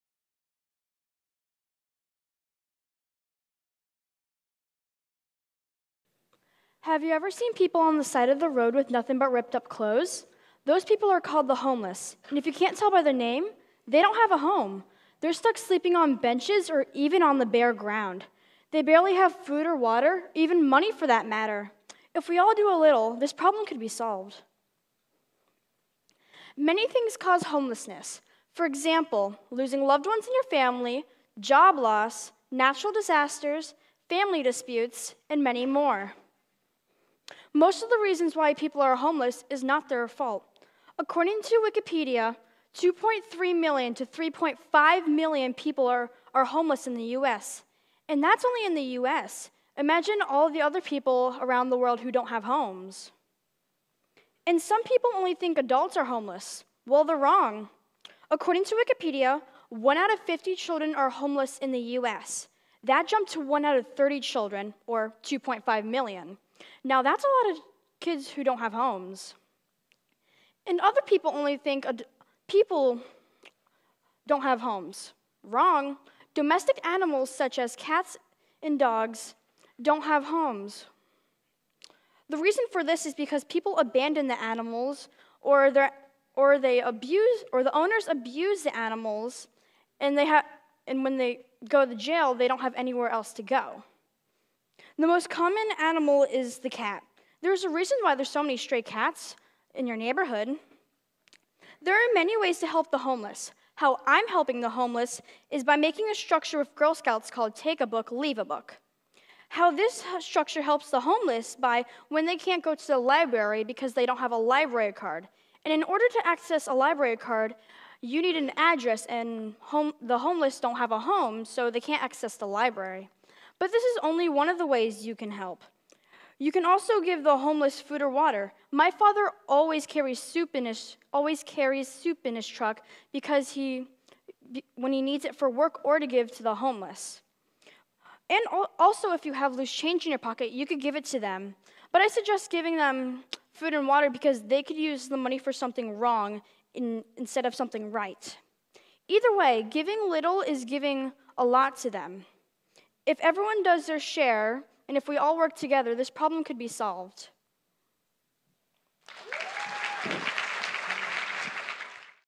Homelessness | Pearl Ann Schmidt | TEDxPascoCountySchools
Pearl Ann Schmidt discusses the negative effects faced by people who are homeless. Student This talk was given at a TEDx event using the TED conference format but independently organized by a local community.